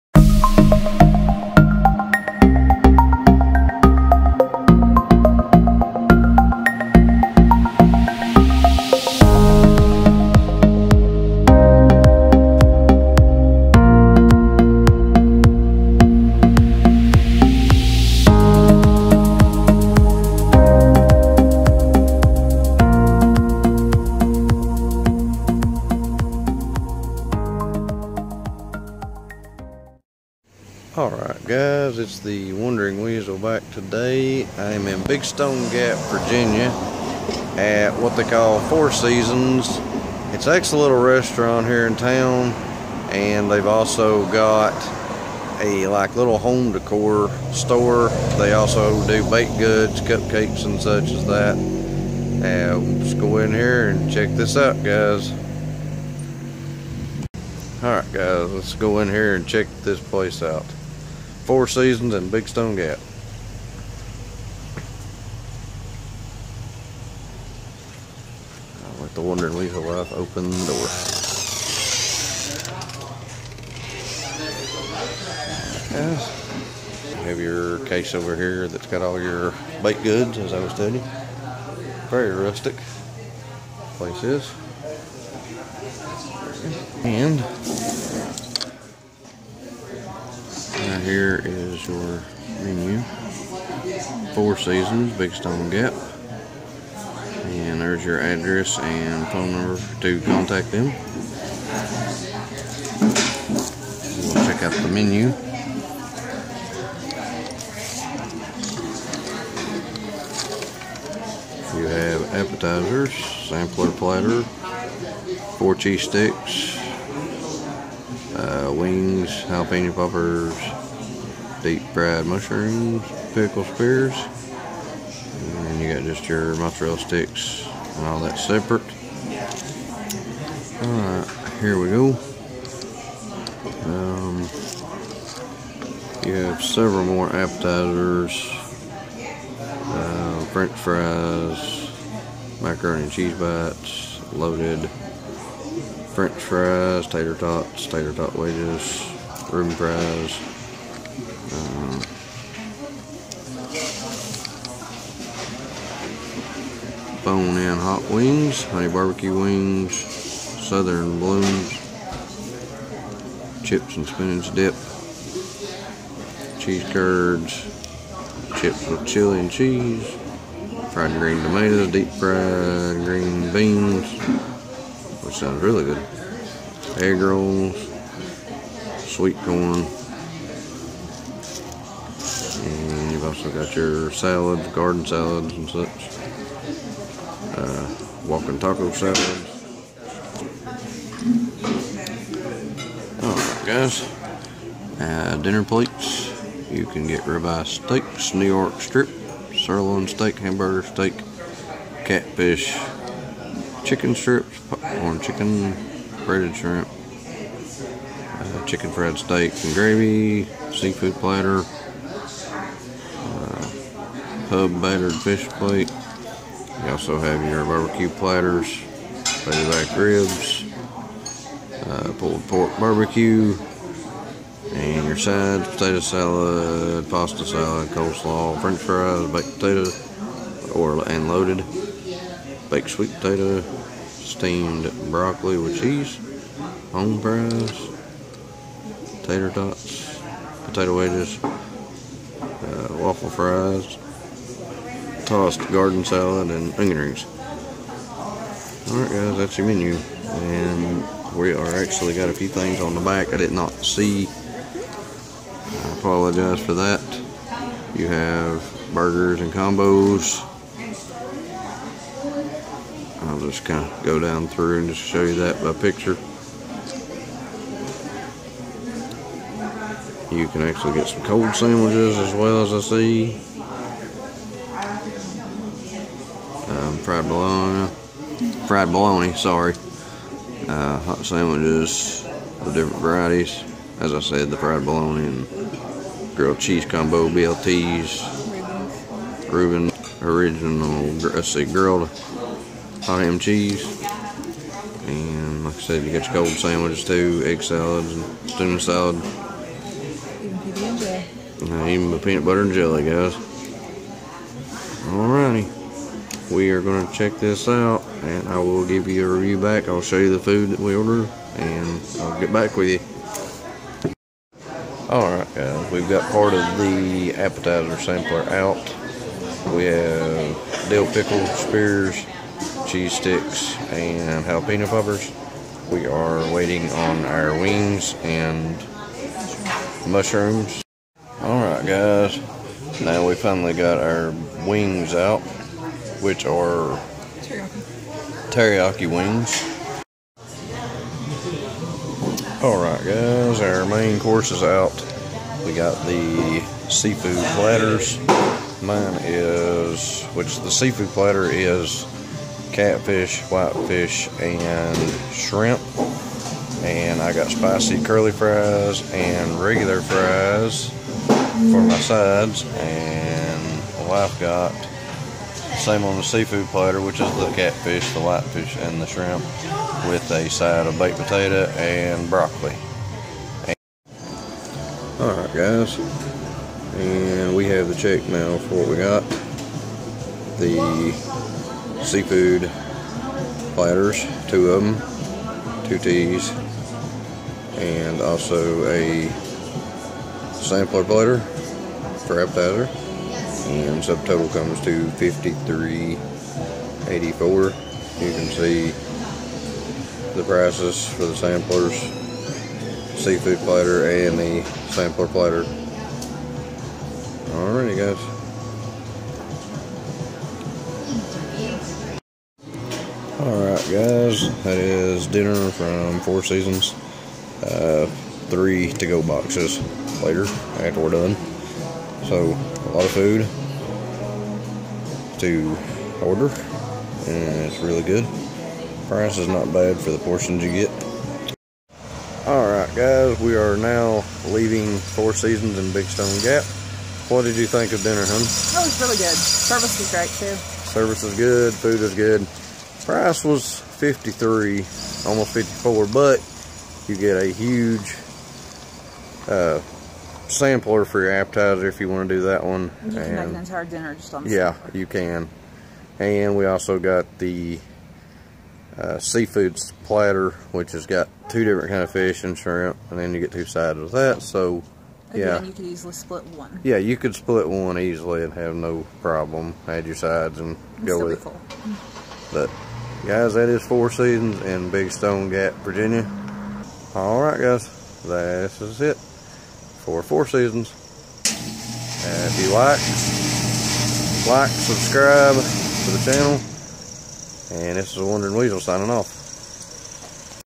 Four Seasons Restaurant
Join me as we try Four Seasons restaurant in Big Stone Gap Virginia.